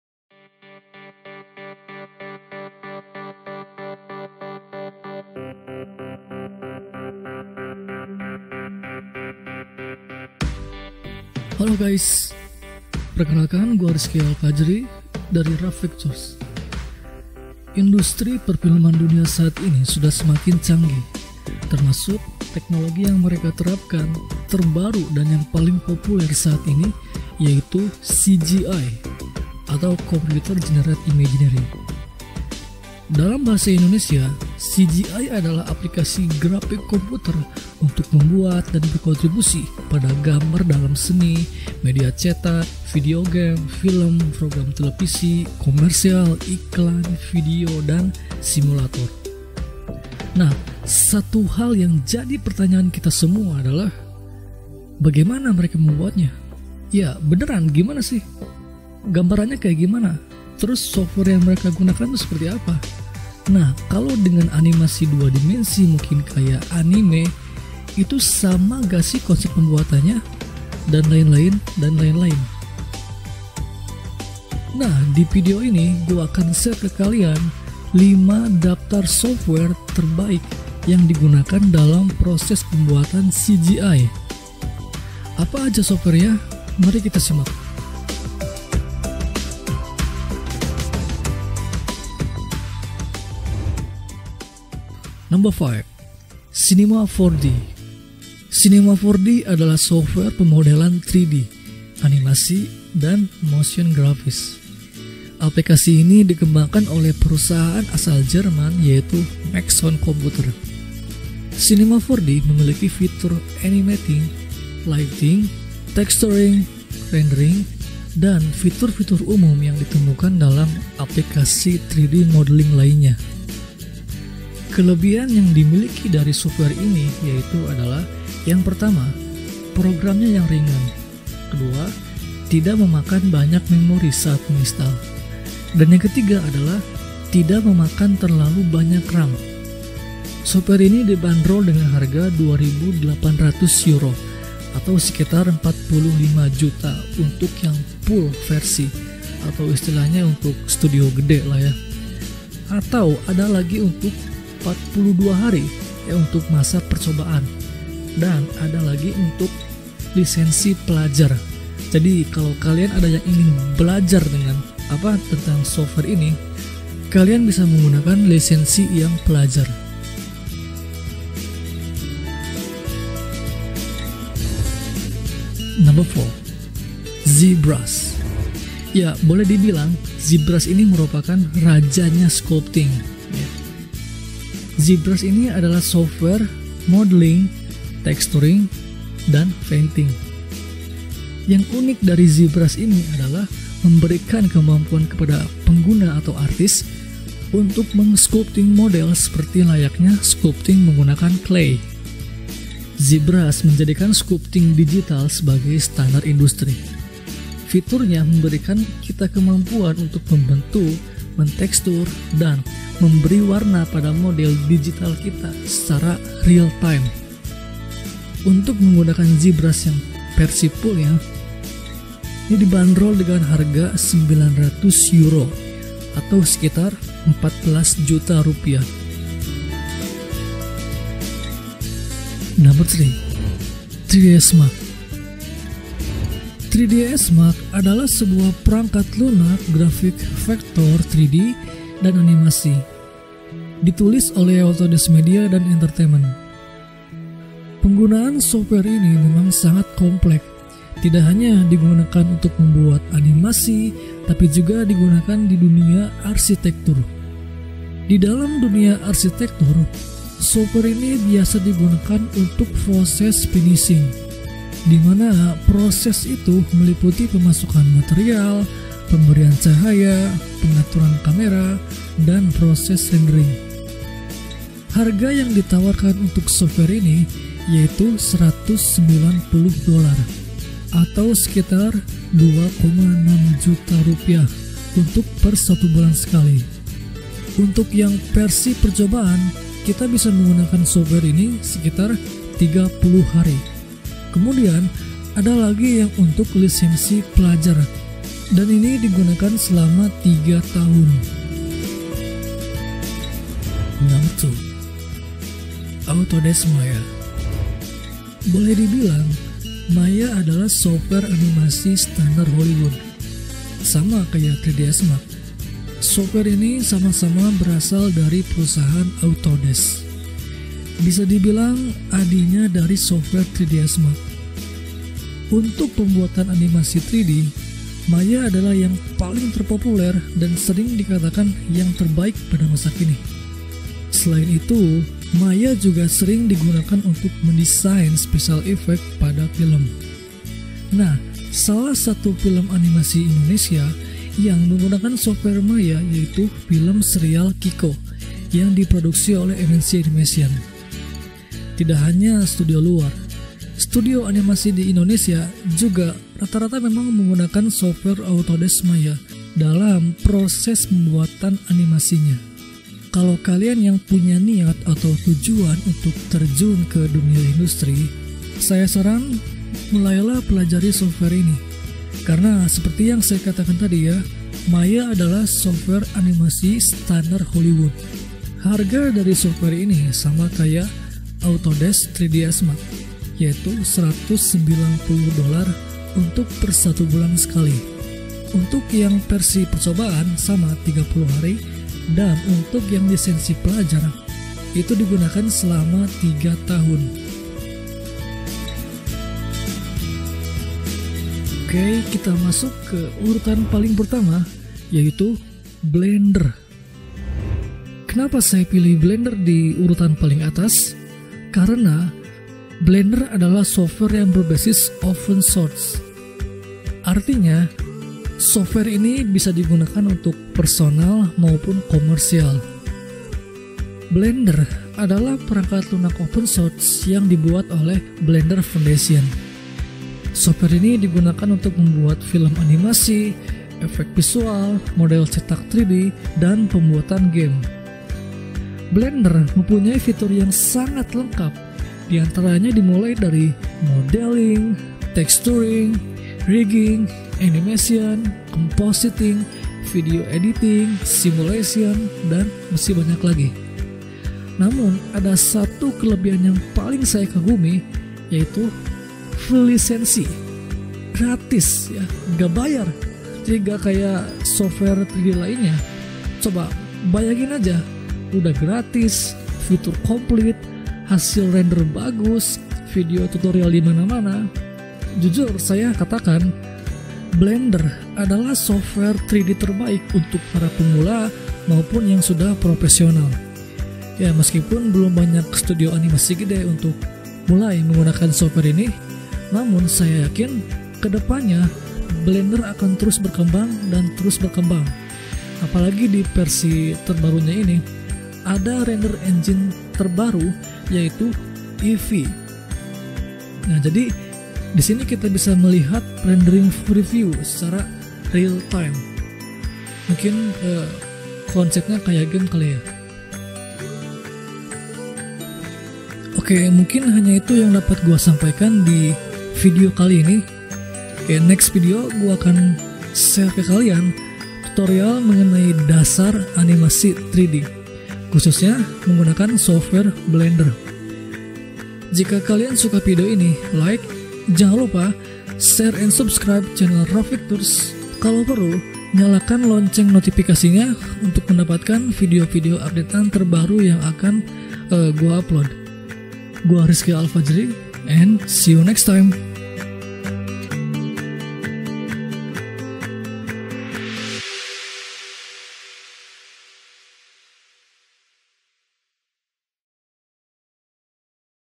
5 SOFTWARE TERBAIK UNTUK PEMBUATAN CGI
Teknologi dalam dunia perfilman sudah semakin canggih. Untuk mendapatkan satu scene atau adegan yang membutuhkan efek khusus seperti ledakan, laser, api atau menambahkan robot dan monster kedalam adegan dibutuhkan teknologi yang bernama CGI (Computer Generate Imaginary). Berikut adalah 5 daftar software yang sering digunakan dalam proses pembuatan CGI :

1. Cinema 4D by Maxon

2. zBrush by Pixologic

3. 3Ds Max by Autodesk

4. Maya by Autodesk

5. Blender by Blender Foundation